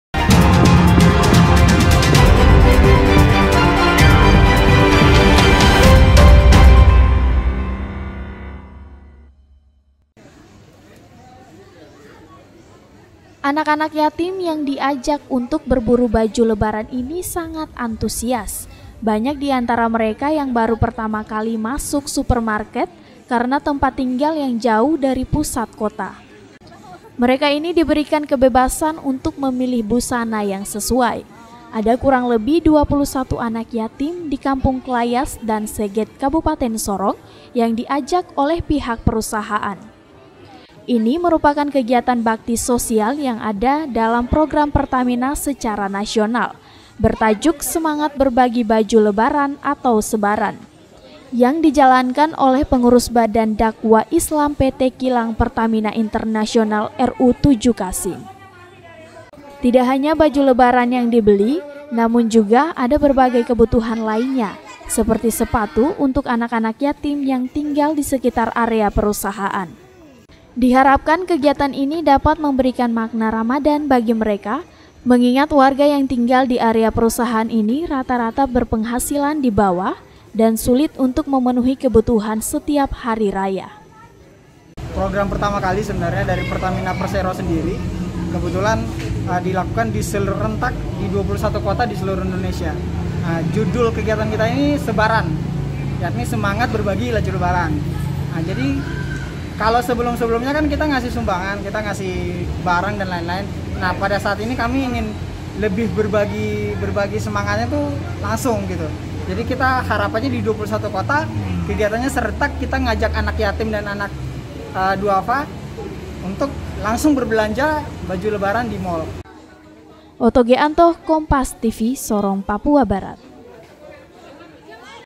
PT Kilang Pertamina Internasional RU VII Kasim Ajak Anak Yatim Berburu Baju Lebaran
SORONG, KOMPAS.TV - Ada berbagai cara yang dapat dilakukan untuk membantu masyarakat yang tidak mampu pada bulan ramadan, seperti yang dilakukan PT Kilang Pertamina Internasional Ru Vii Kasim yang mengajak anak-anak yatim di wilayah operasi perusahaan untuk berburu baju lebaran.

Anak-anak yatim yang diajak untuk berburu baju lebaran ini sangat antusias, banyak diantara mereka yang baru pertama kali masuk supermarket karena tempat tinggal yang jauh dari pusat kota. Mereka ini diberikan kebebasan untuk memilih busana yang sesuai. Ada kurang lebih 21 anak yatim di kampung Klayas dan Seget kabupaten Sorong yang diajak oleh pihak perusahaan.

Ini merupakan kegiatan bakti sosial yang ada dalam program Pertamina secara nasional, bertajuk semangat berbagi baju lebaran atau sebaran, yang dijalankan oleh pengurus badan dakwah islam PT Kilang Pertamina Internasional Ru Vii Kasim.

Tidak hanya baju lebaran yang dibeli namun juga ada beberapa kebutuhan lainnya seperti sepatu, untuk anak-anak yatim yang tinggal disekitar area perusahaan.

Diharapkan kegiatan ini dapat memberikan makna ramadan bagi mereka, mengingat warga yang tinggal di area perusahaan ini rata-rata berpenghasilan dibawa dan sulit untuk memenuhi kebutuhan setiap hari raya.